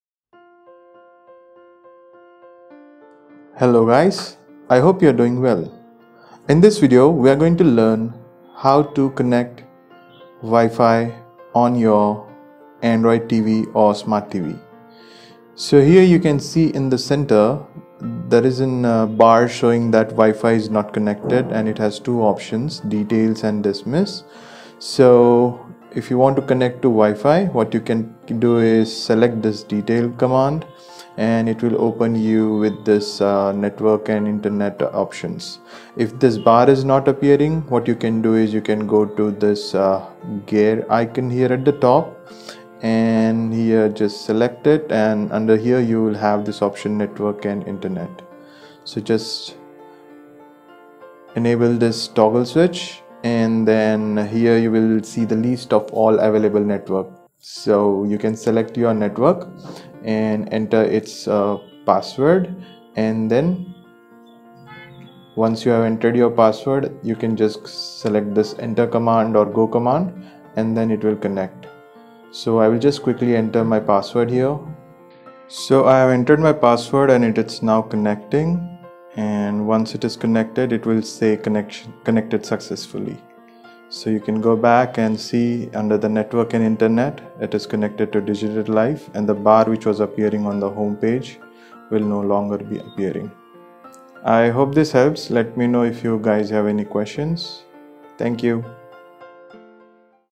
How to Connect Android TV with a Wi-Fi Network
In this tutorial, I have shared how you can connect your Android Smart TV to the internet using the available Wi-Fi connection.

After connecting to the network, the smart TV will be using the internet to browse and stream the video.